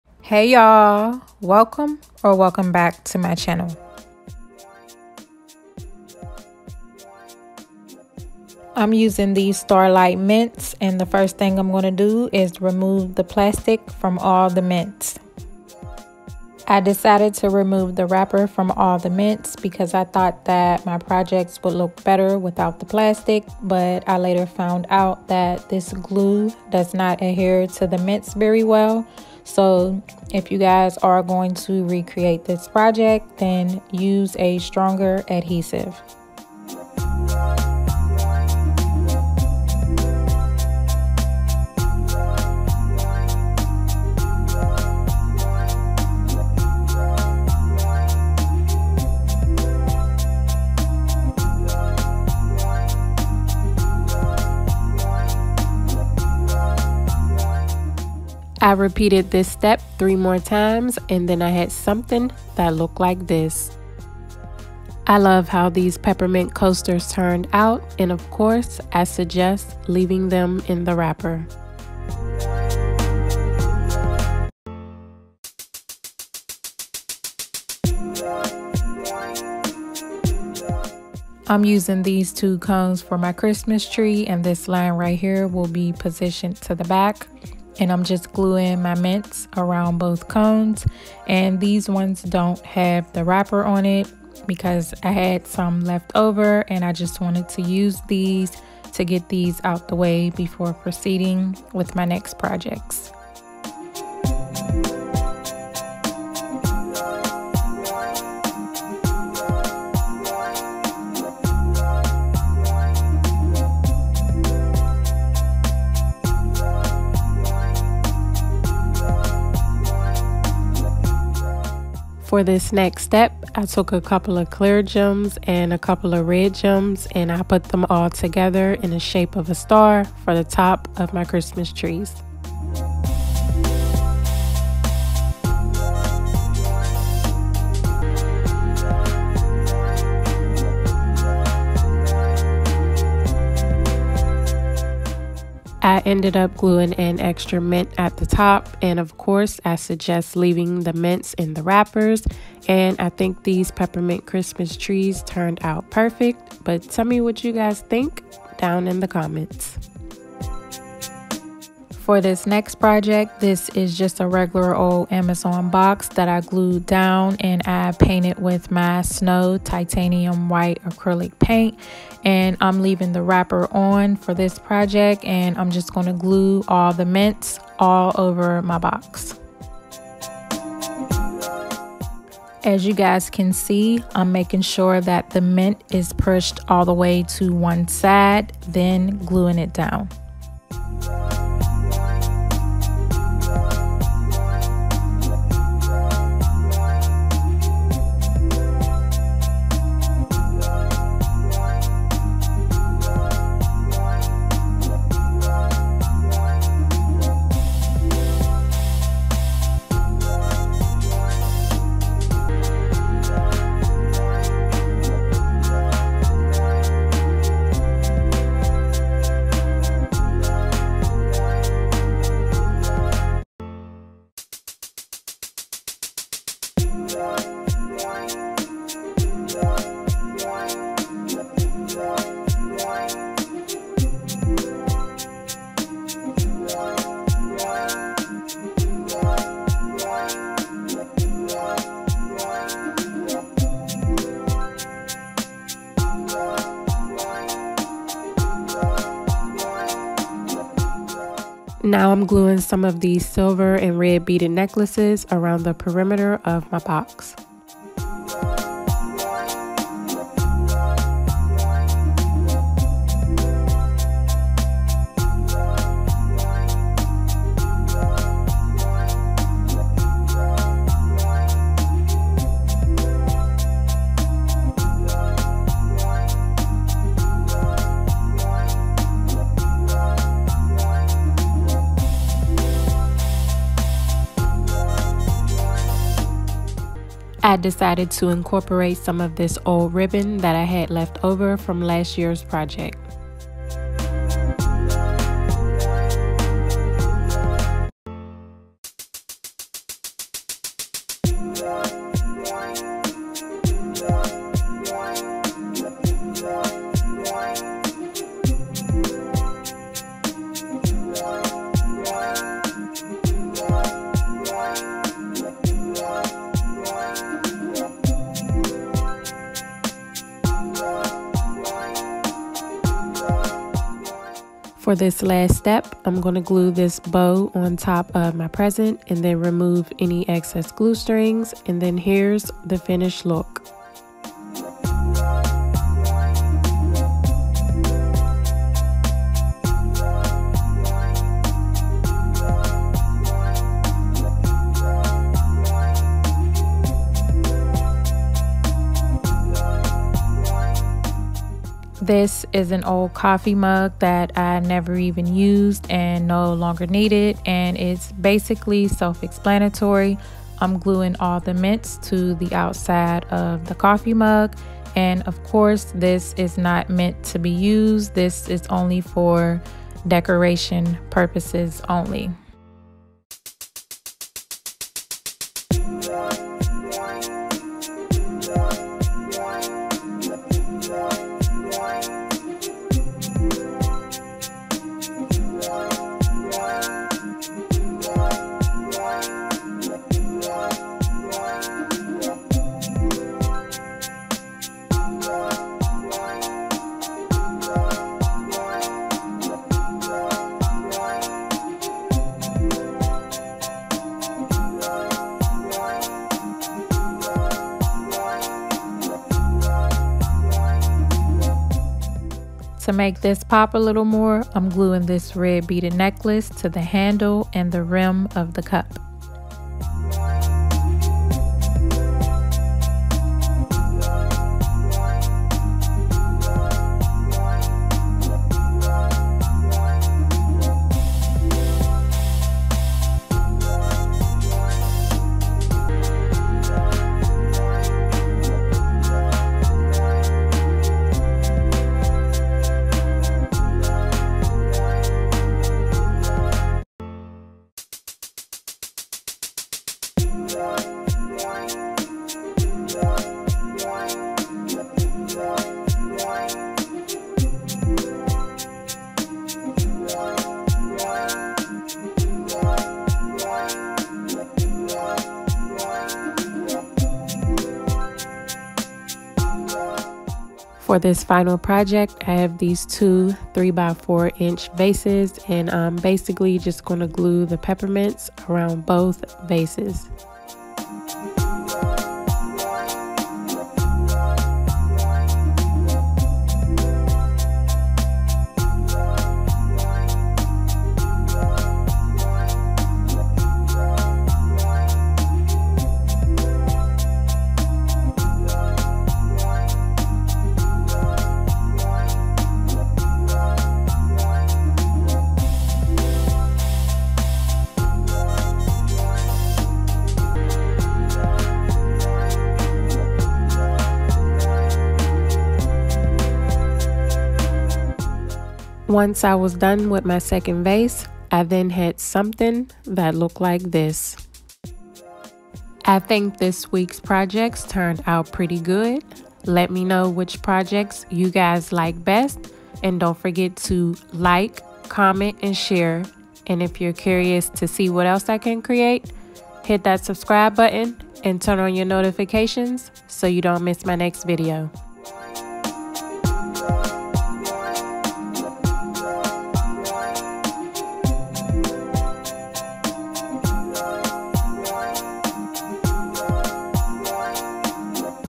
Cool Gifts To Make With Mints || Christmas Gifts With Mints || Dollar Tree DIY’s
Hey y'all! Check out these cool gifts to make with mints DIY Projects. These Dollar Tree Mint DIY's were fun and easy to make and very budget friendly. They can be used for Holiday Christmas Decor around your home, for your desk at the office, parties, events, (etc)... Let me know what y'all think about these cool gifts I made down in the comments.

**F.Y.I IF YOUR GOING TO RECREATE THIS PROJECT USE A STRONGER ADHESIVE SO IT CAN LAST OVERTIME**

✨ITEMS USED ⬇️

***ALL ITEMS USED ARE FROM DOLLAR TREE***

Mints, Beaded Necklaces, Candy Canes, Bow, Ribbons, Cones, Red & Clear Gems, Vases, LED Lights, Ruler, (Dollar Tree)

Box (Amazon)

Snow White Acrylic Paint (Michaels)

Hot Glue Gun/Glue Sticks (Dollar General)